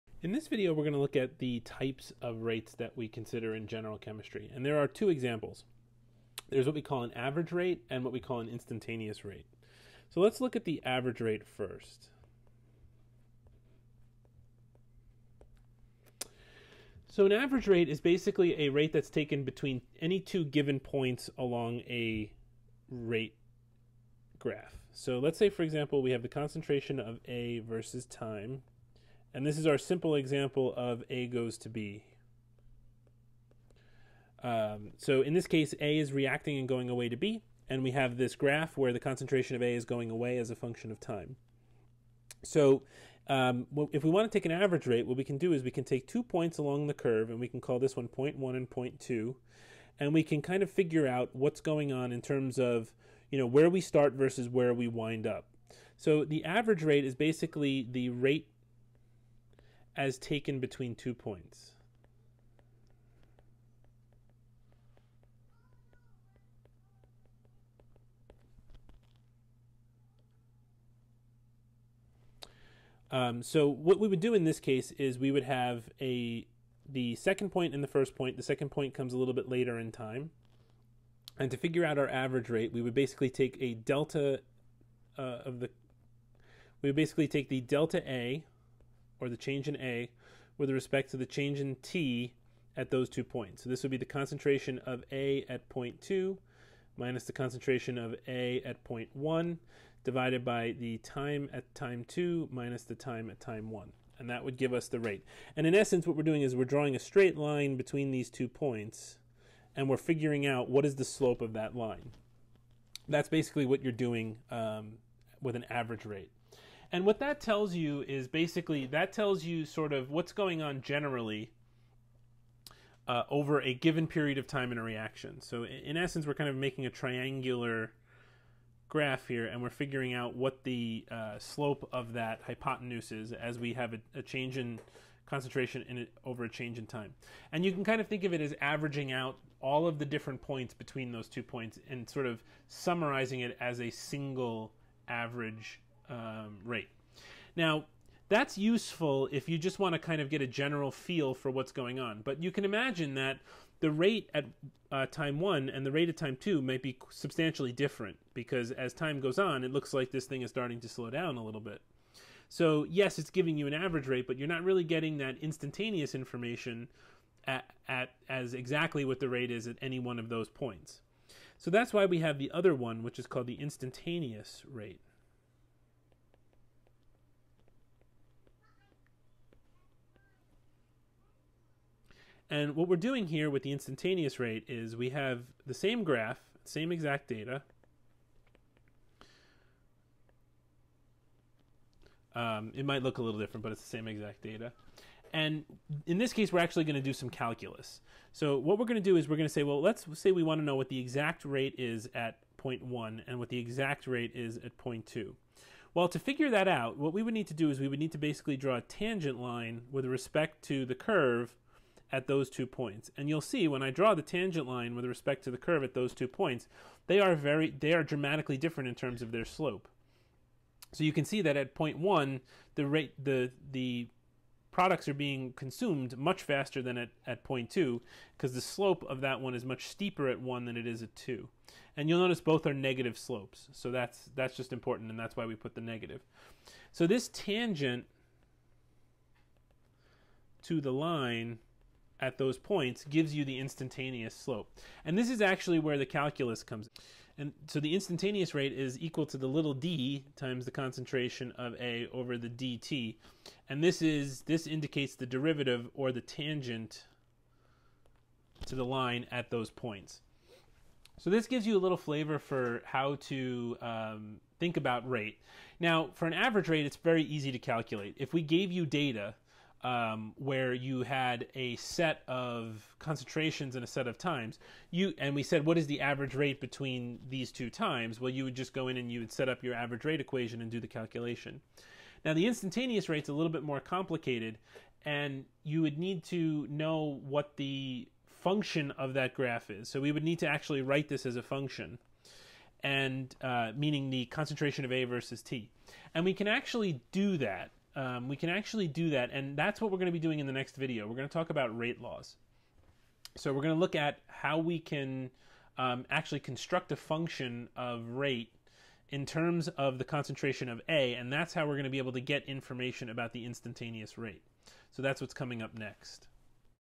Chapter 13. Average vs. Instantaneous Rates
In this video, we explore two different ways of quantifying reactions: one involving the average rare between two given times during a reaction and the other the instantaneous rate a specific time during the reaction.